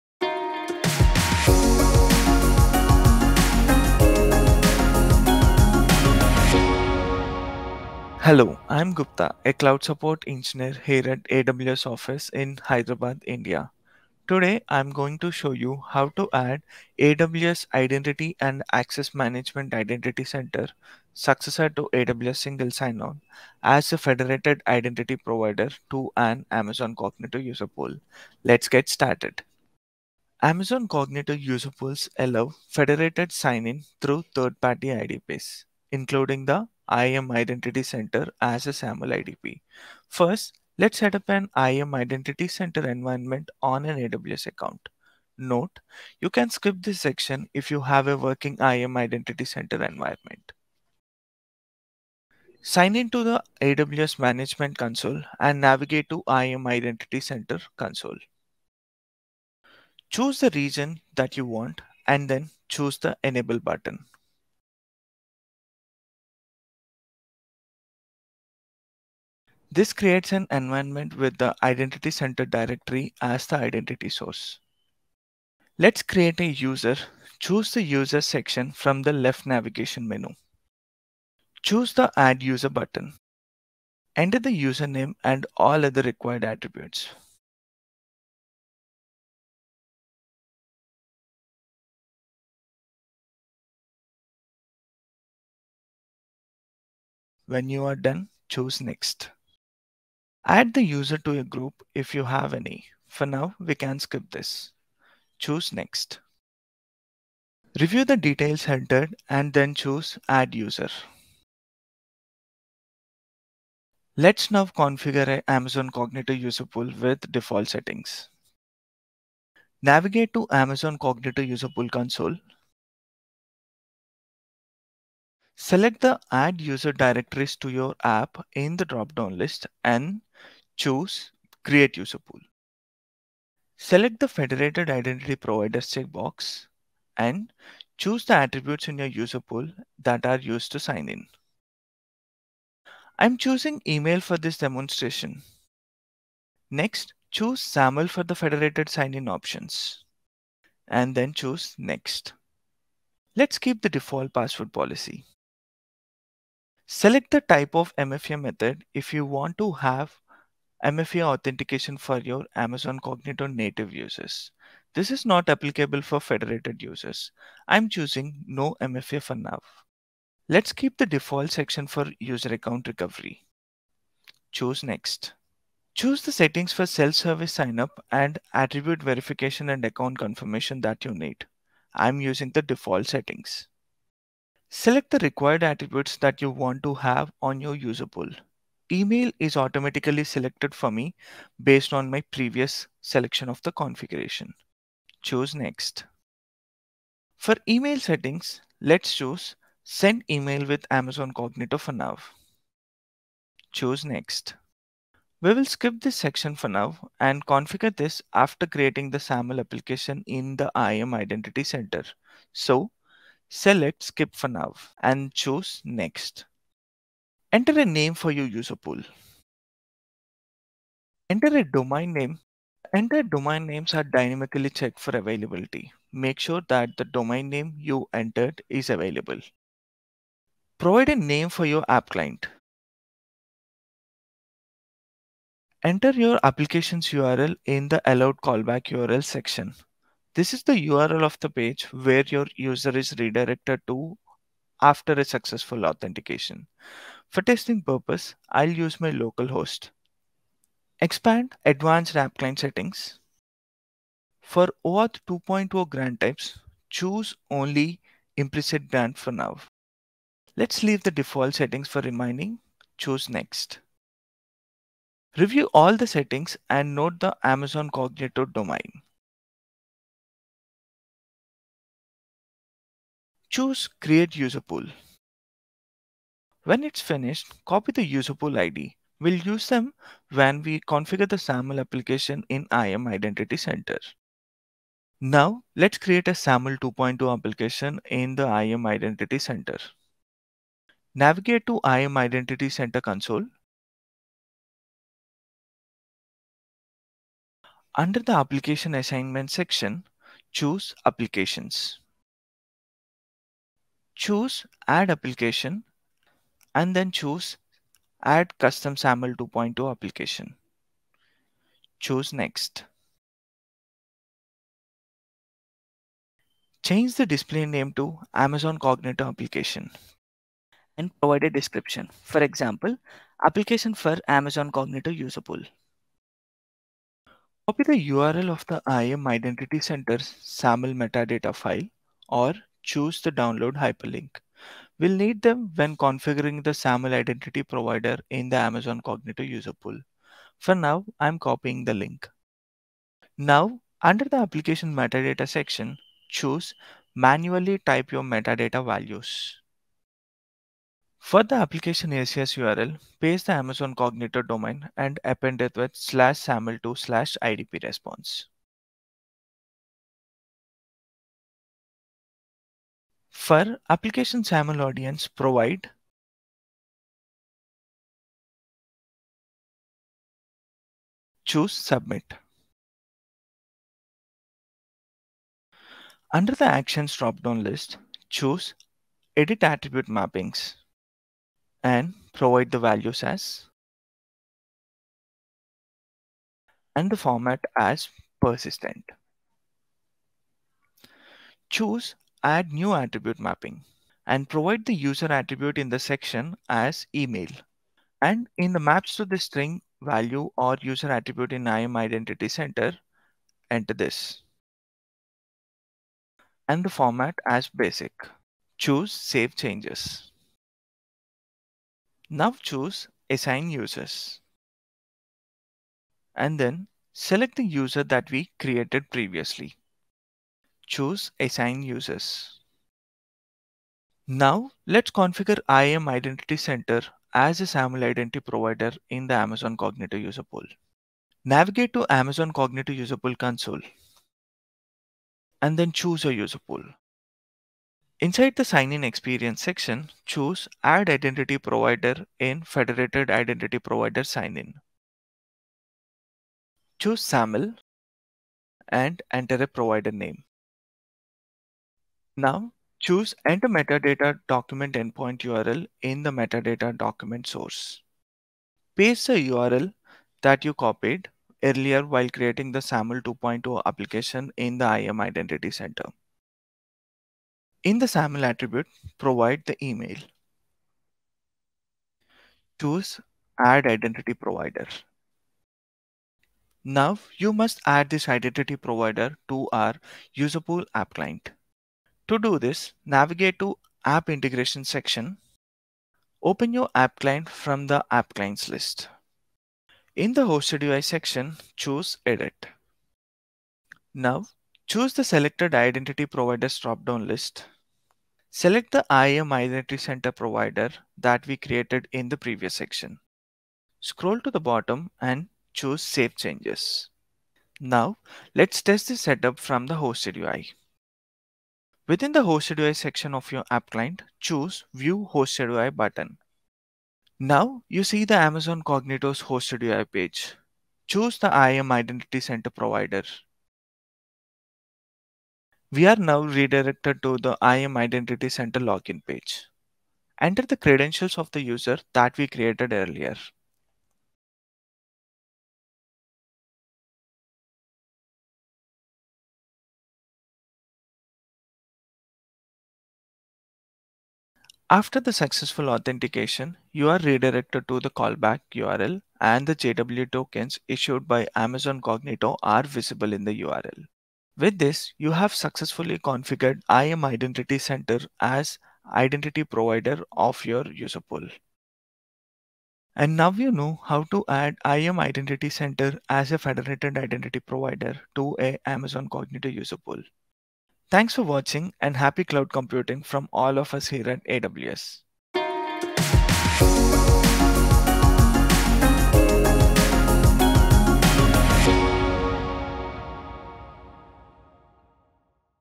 How do I integrate IAM Identity Center with an Amazon Cognito user pool?
Skip directly to the demo: 0:52

For more details on this topic, see the Knowledge Center article associated with this video

Gupta shows you how to integrate IAM Identity Center with an Amazon Cognito user pool.

0:00 Introduction
0:52 Demo Begins
1:53 Configure a Cognito User Pool with Default Settings
4:44 Create a SAML2.0 Application in IAM Identity Center
7:03 Configure IAM Identity Center as a SAML Identity Provider in Cognito User Pool
7:56 Add Identity Provider to your User Pool App Client
8:27 Testing the Set Up from the Hosted UI
9:22 Closing